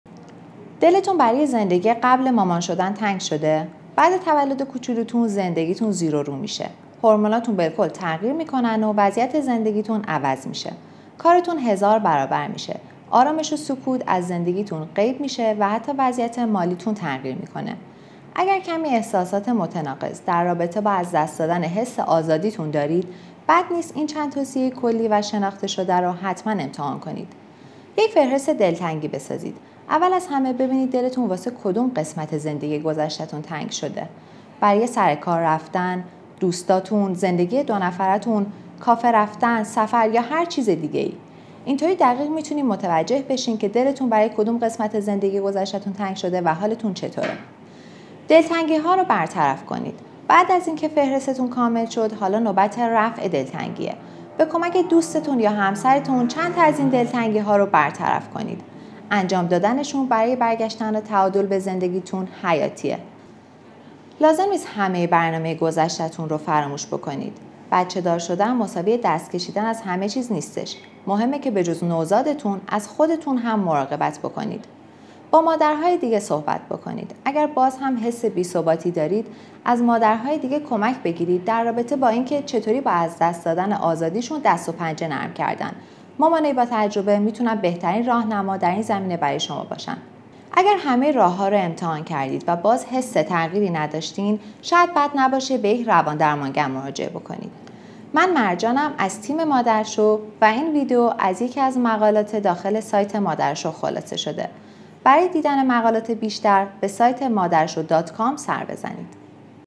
بازگشت به زندگی قبل از بارداری
اگر شما هم دلتون برای زندگی قبل از بارداری و بچه‌دار شدن تنگ شده اسن ویدیو رو از دست ندین.
تو این ویدیو براتون چندتا راهکار داریم تا زندگی‌تون به زندگی قبل از بارداری و بچه‌دار شدن نزدیک‌تر بشه.